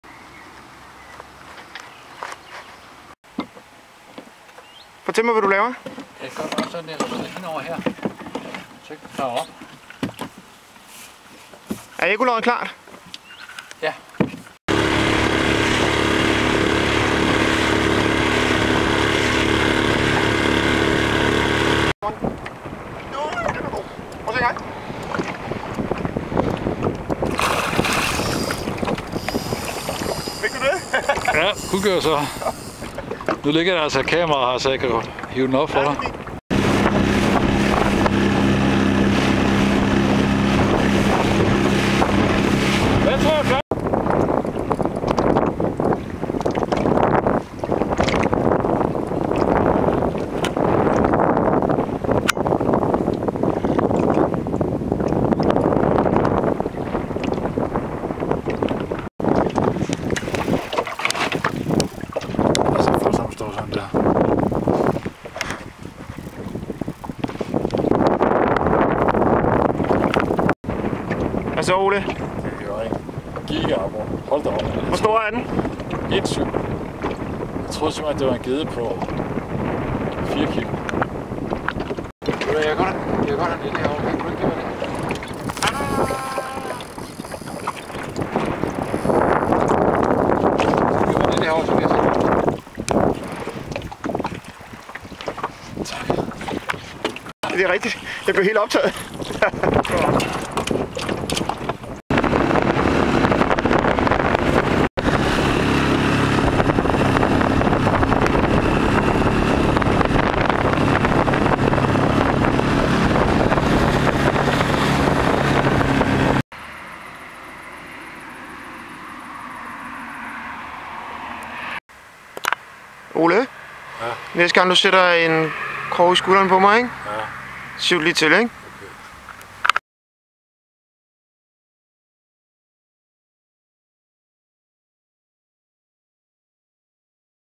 God gedde og store aborrer på Tissø, juli/2011
Holbæk og Omegns Lystfiskerforening er netop trådt ind i fællesskabet omkring to både i Tissø. 3-4 af klubbens medlemmer har fundet et par gode spots. TJek denne video, som byder på et par seriøse aborrer.: 1,7 og 1 kilo.